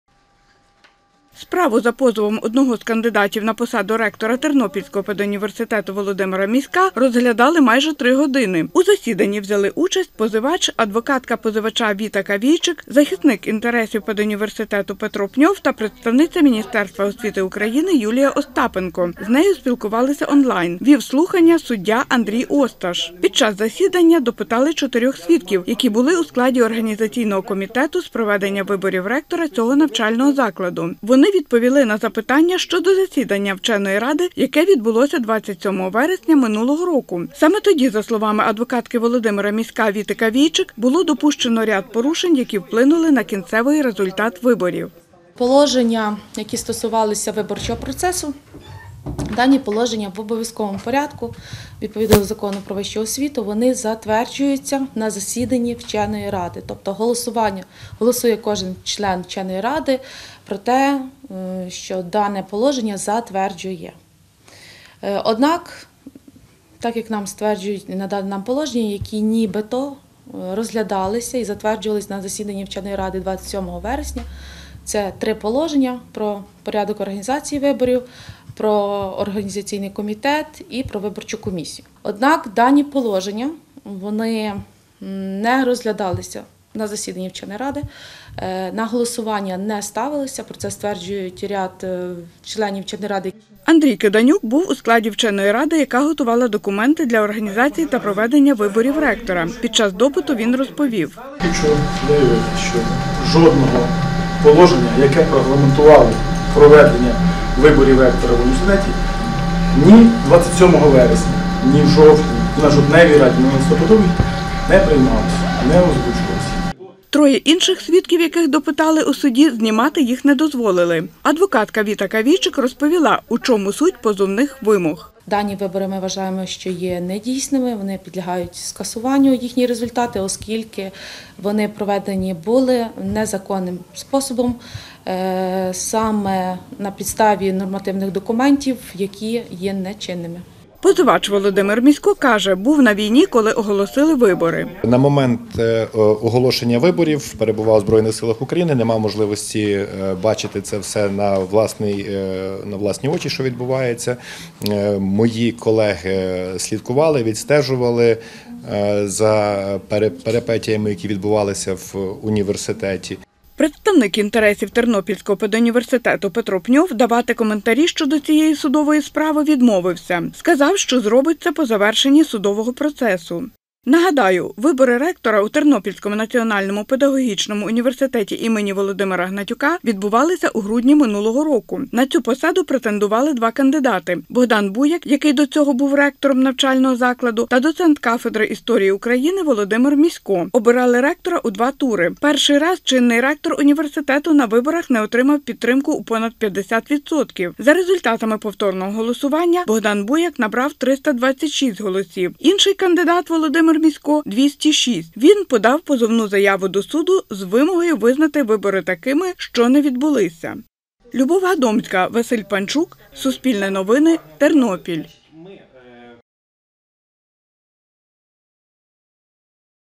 Кандидат на посаду ректора судиться через вибори
В окружному адміністративному суді допитали свідків у справі за позовом доцента кафедри історії України Тернопільського педуніверситету Володимира Міська. Чоловік був одним з кандидатів на посаду ректора цього навчального закладу. За результатами голосування, на виборах переміг попередній ректор Богдан Буяк. Володимир Місько подав позовну заяву до суду з вимогою визнати вибори такими, що не відбулися.